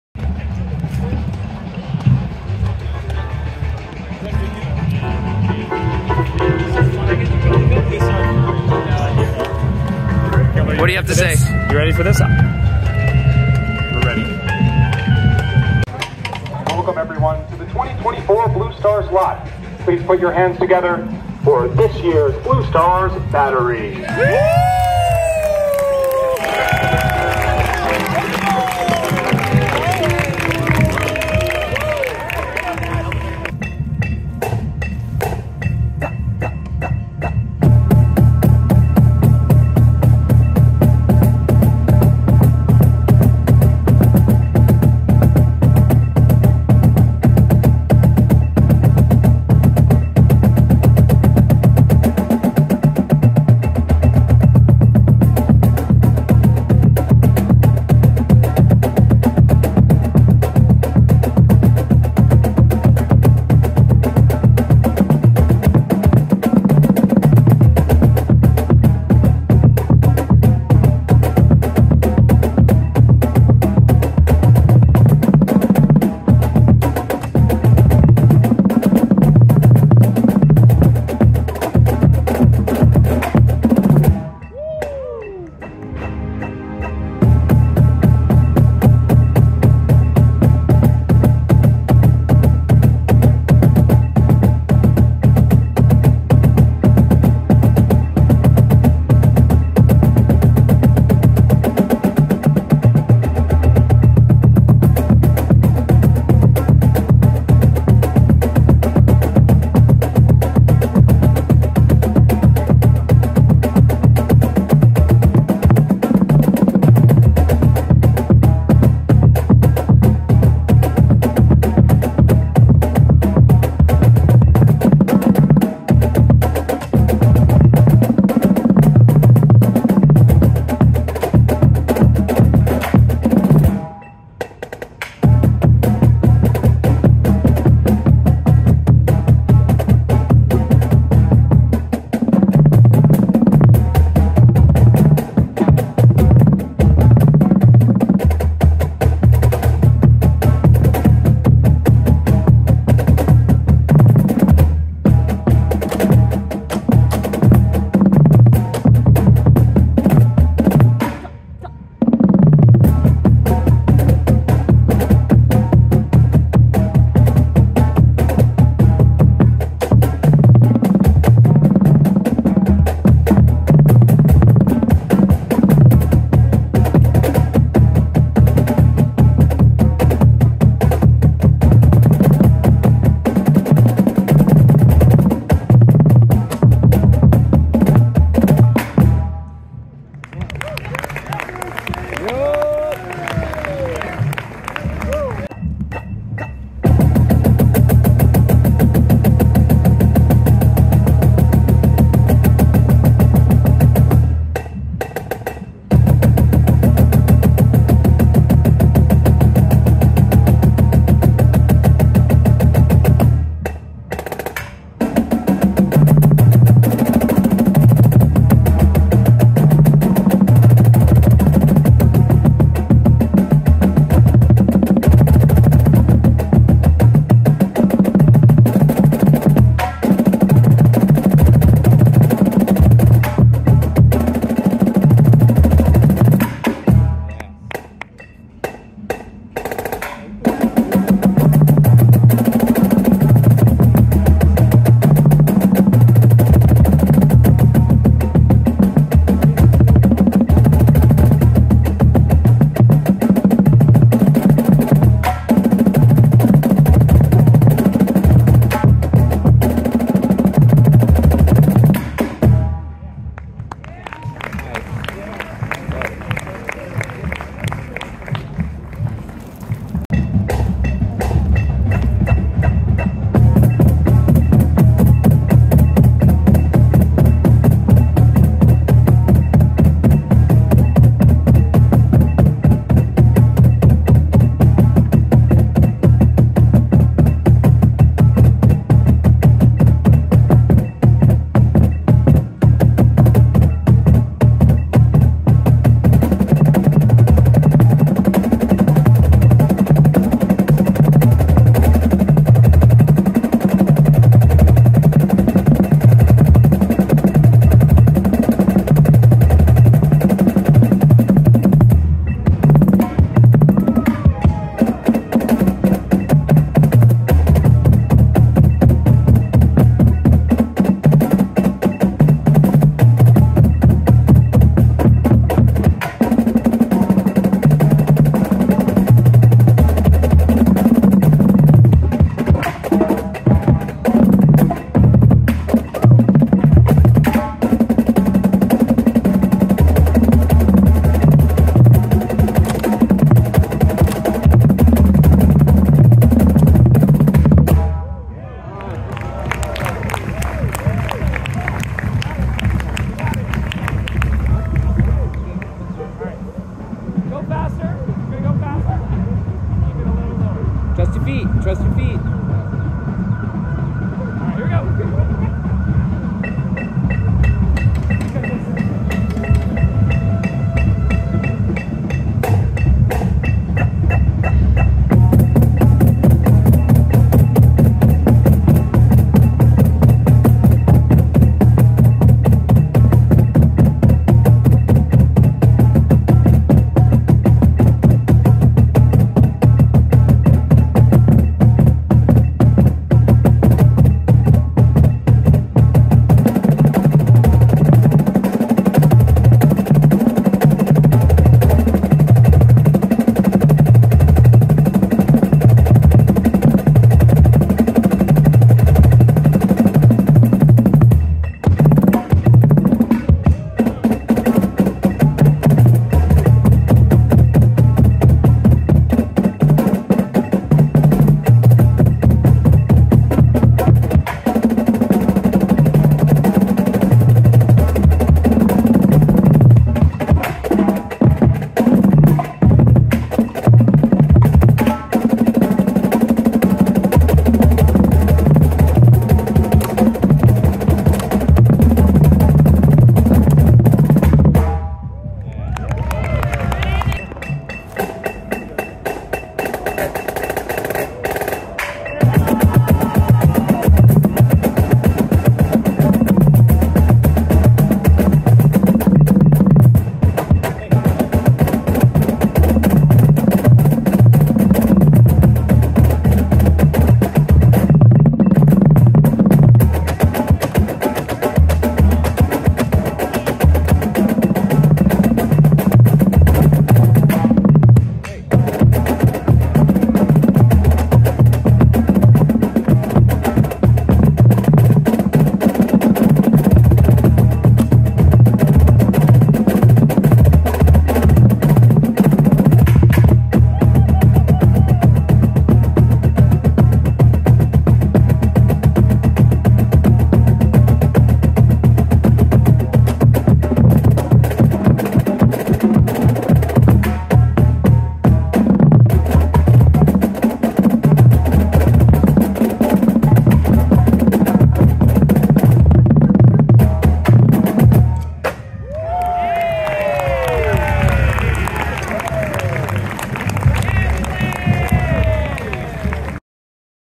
Blue Stars Bass 2024 Finals Warm Up (Ant POV)
Finals Day at Military Park. It was an honor working with these people. Obviously from the POV of an ant, as you can see from the grass.
FCO.

Timestamps:
0:00 Introduction
0:35 Multi-Bounce
2:15 ATTwitch
3:16 Singles
4:31 Paradiddles
7:28 Wii Flams
8:42 Fin